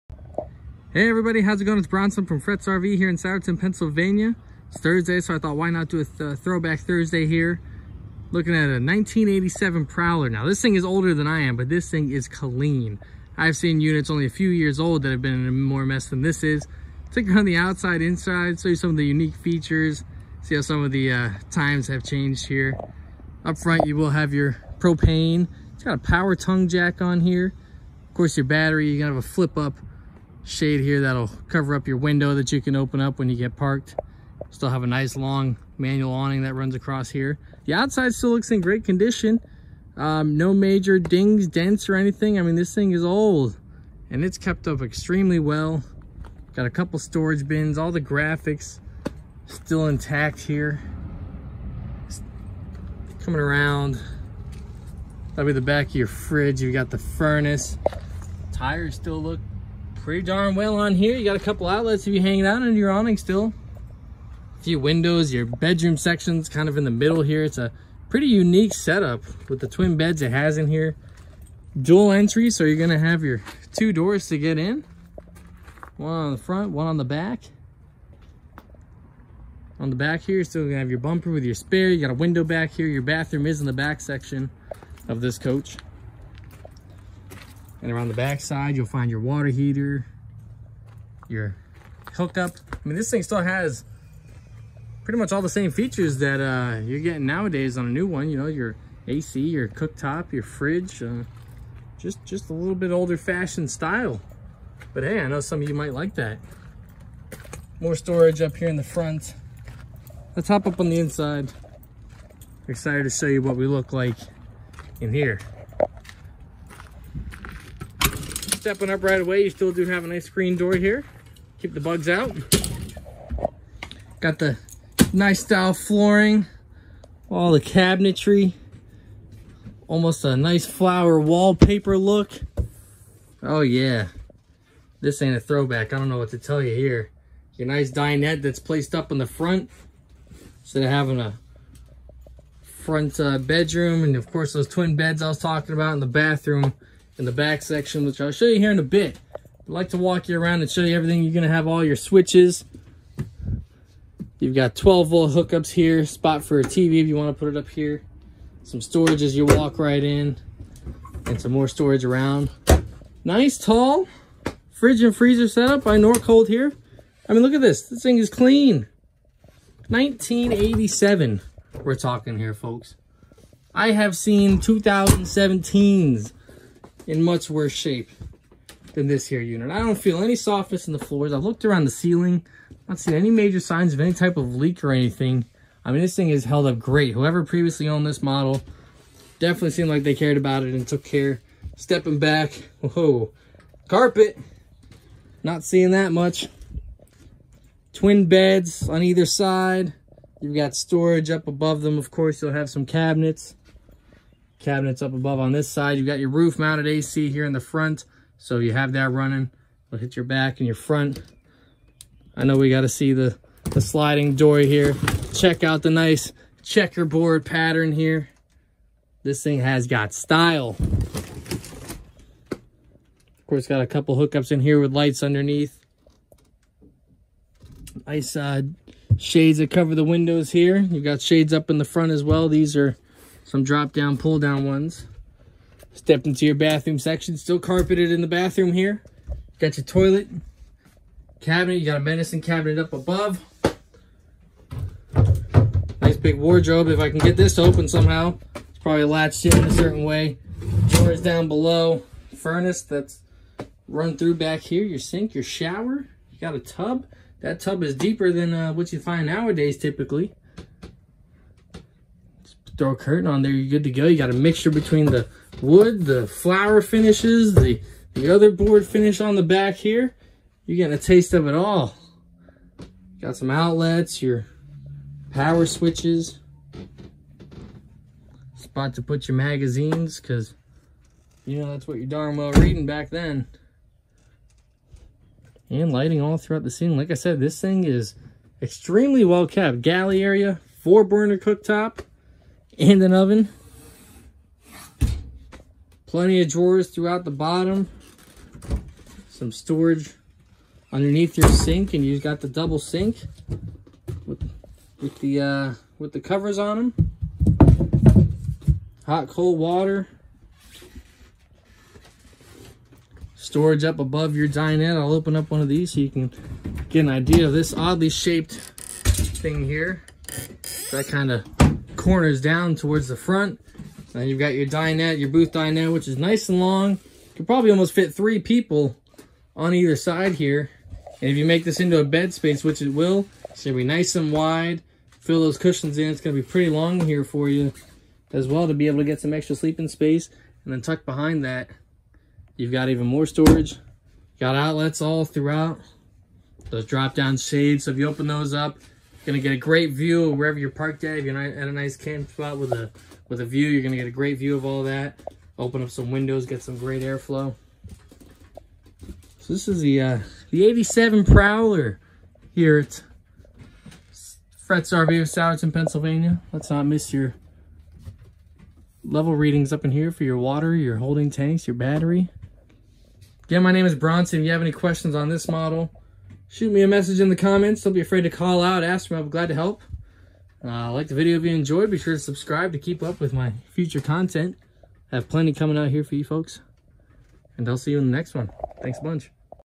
1987 Fleetwood Prowler! Throwback Thursday
Here is a walkthrough of the 1987 Fleetwood Prowler 25N.